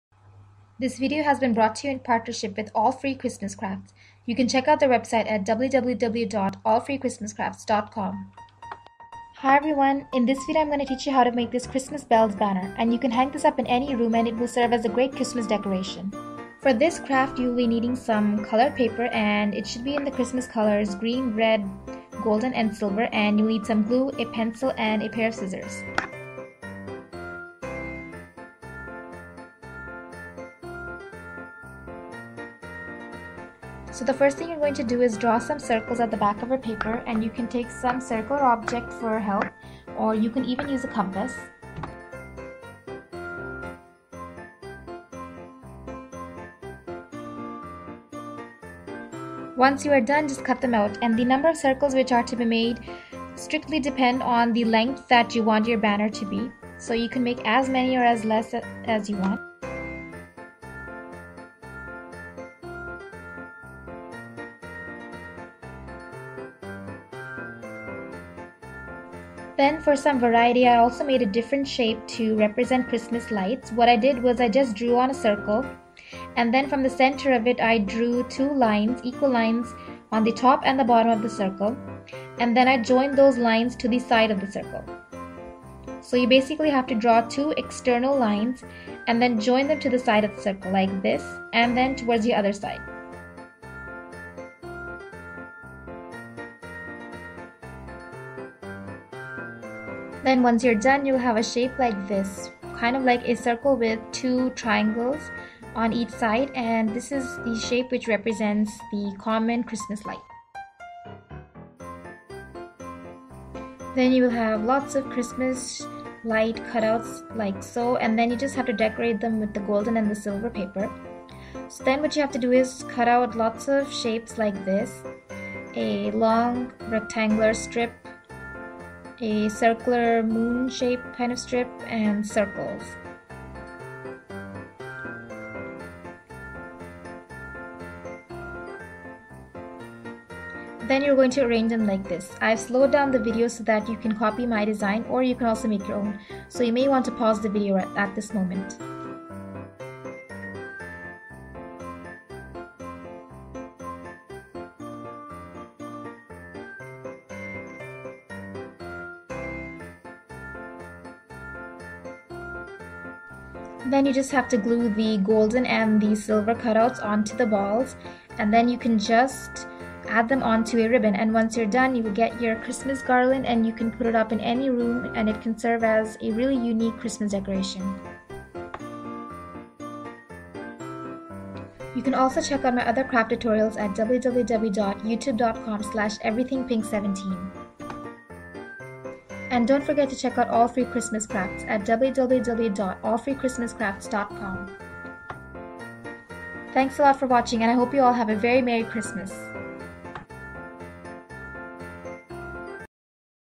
How to Make a Sparkling Paper Ornaments Banner
Decorating for Christmas doesn't have to be complicated; just look at how easy it is to make a Sparkling Paper Ornaments Banner. Grab your favorite festive and shiny holiday paper and follow this simple tutorial. In minutes, you'll have a new homemade Christmas decoration that you can put anywhere in the house and store easily. You can have fun with it and change up the ornament designs as often as you like.